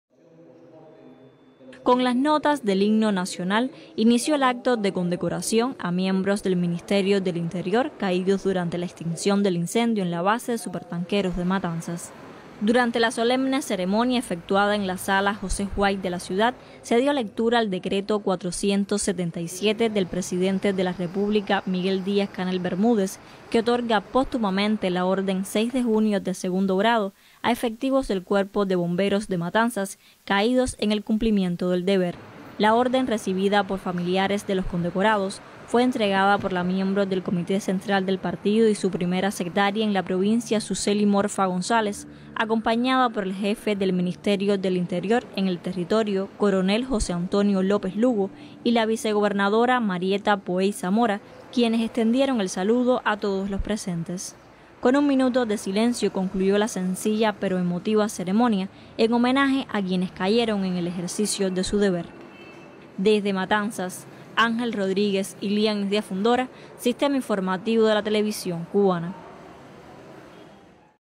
Familiares de cubanos fallecidos en incendio de Matanzas reciben medallas
Los familiares de los cubanos de Matanzas fallecidos en el incendio de la Base de Supertanqueros recibieron este viernes las medallas póstumas en un acto solemne, informó el canal oficialista TV Yumurí.

Los condecorados fueron Diosdel Nazco Vargas, primer suboficial de 39 años; Leo Alejandro Doval del Prado, soldado de 19 años; Adriano Rodríguez Gutiérrez, soldado de 18 años; Fabián Naranjo Núñez, soldado de 19 años; Luis Ángel Álvarez Leyva, de 24 años, y Pablo Ángel López Martell, informó la Agencia Cubana de Noticias (ACN).

En las imágenes emitidas por TV Yumurí, los familiares aparecen llorando cuando reciben las medallas póstumas en "una breve pero emotiva ceremonia" escenificada en la Sala White.

Según la citada fuente, "mediante el decreto presidencial 477, las máximas autoridades del Partido Comunista de Cuba (PCC) y el Gobierno en la provincia de Matanzas condecoraron hoy póstumamente, con la Orden 6 de Junio de Segundo Grado, a seis de los caídos en el incendio de grandes proporciones".

Video: TV Yumurí.